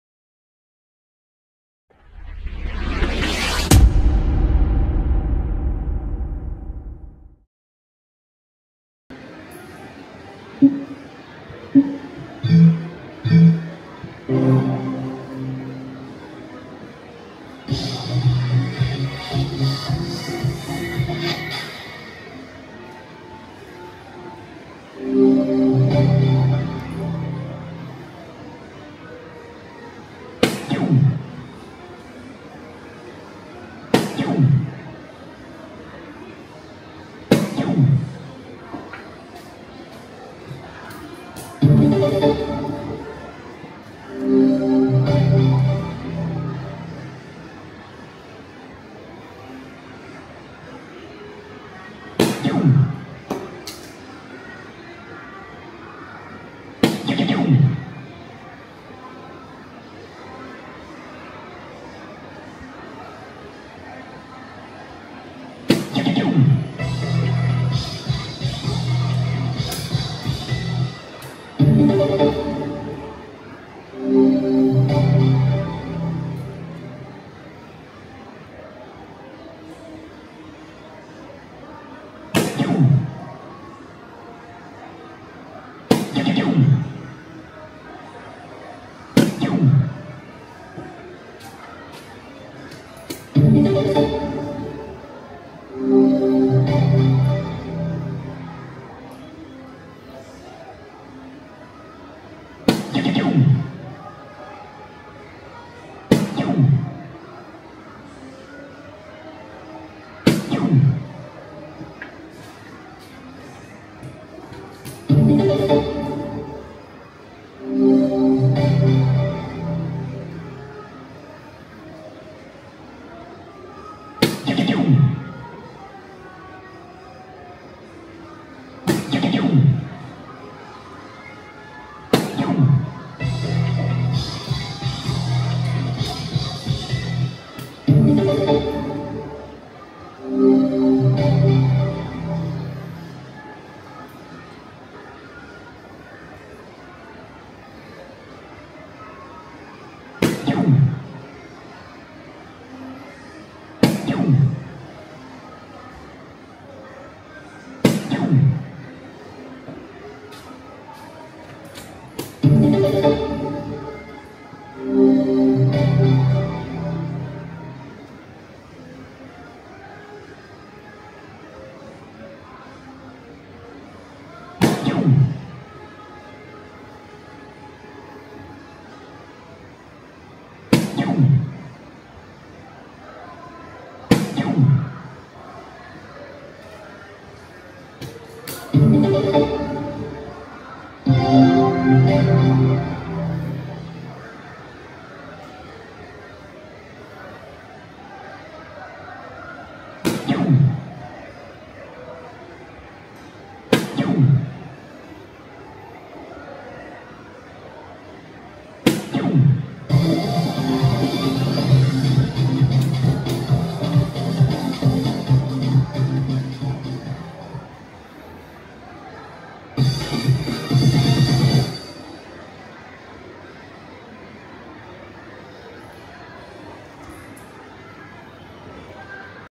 【darts】1000points Challenge "T20" #720
動画を観ていただきありがとうございます。応援していただけると嬉しいです💕😆👍

🎯【今回のチャレンジ】
▶︎count-upで1000点目指します!!［T20編］